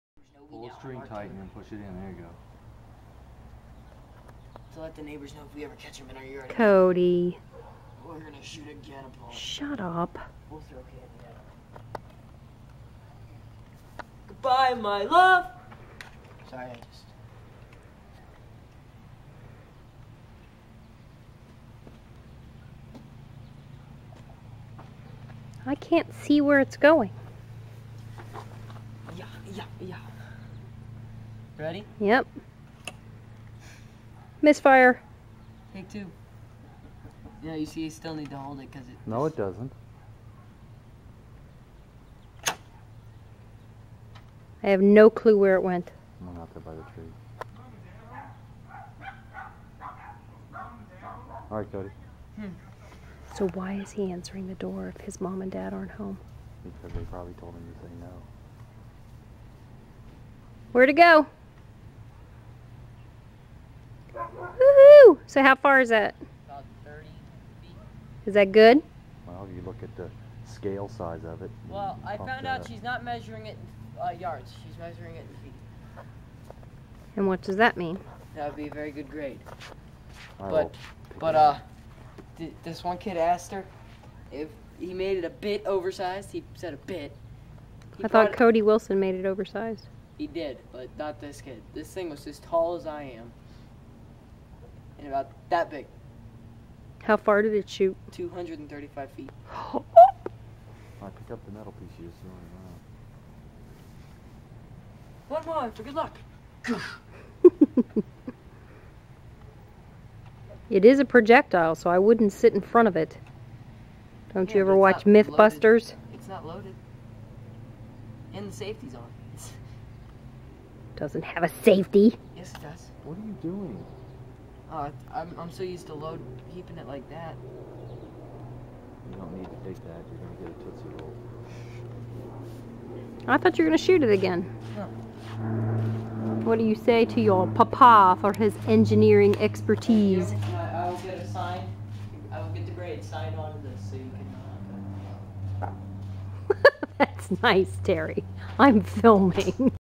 9th Grade Catapult Project
So, he took his Catapult to school for his last day of 9th grade.. science project. He set it on top of a trash can in the hallway. and it got knocked off. A bunch of guys were horsing around and a water bottle hit the can, thus, catapulting the catapult. After all those trips to Menards, and copious hours of labor to assemble, a wheel got busted off!! :-O It's a good thing I filmed him testing it last night, so we know it DID work. sigh
NOTE #1: Count how many times he has to hitch up his pants (rdrr)
NOTE #2: Be sure to listen for Terry's "special little sound effect" at the end.
NOTE #3: We love our neighbors... We Love Our Neighbors... WE LOVE OUR NEIGHBORS?  ... most of them, anyway.  :-)
NOTE #4: He put the wheel back on with masking tape, and it shot a Tootsie Roll 41 Feet; he got a B.